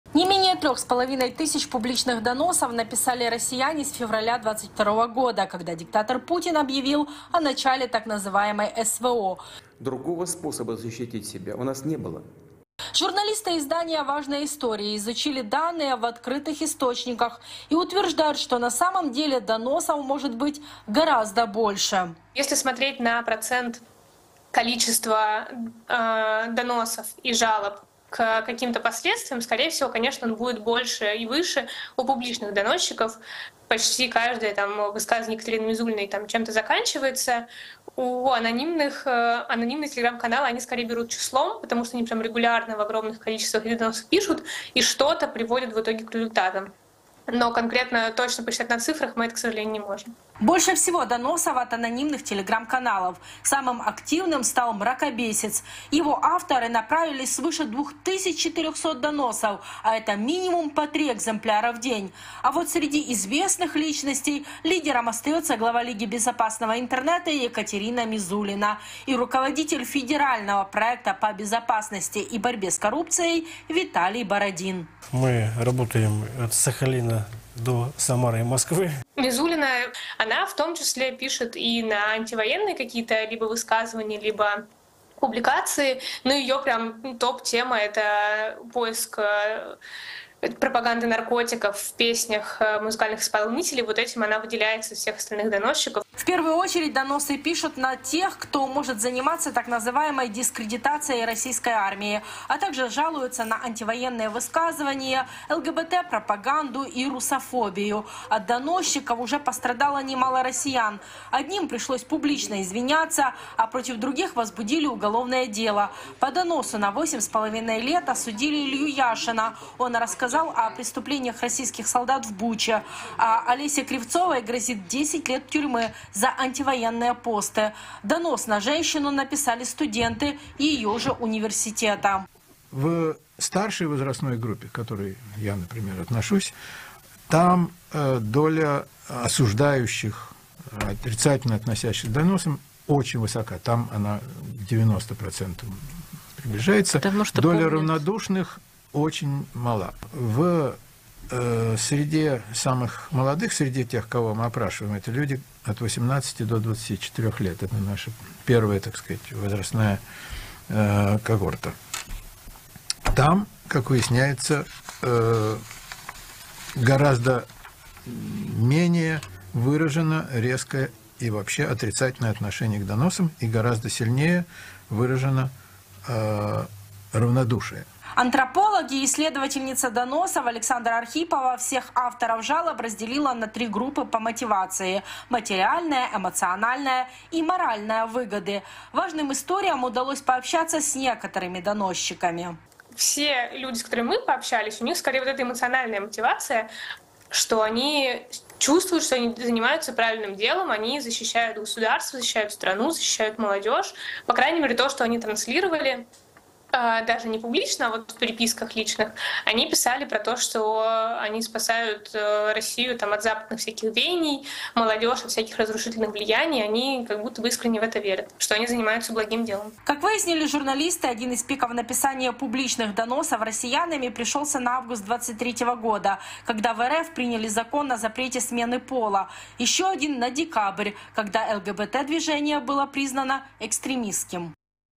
ЭТО ЖЕСТЬ! МАССОВЫЕ ДОНОСЫ в РФ! Мизулина ПОБИЛА ВСЕ РЕКОРДЫ!
В России проверят на "экстремизм" и связи с "иноагентами" игрушки. По мнению чиновников, "неправильные" куклы, мишки и машинки представляют угрозу традиционным ценностям. В нелояльности подозревают даже смайлики и эмодзи в смартфонах. Охота на инакомыслящих в России сопровождается массовым доносительством. В Госдуме уже не справляются с анонимками и предлагают упорядочить процесс подачи жалоб. Подробнее в видео!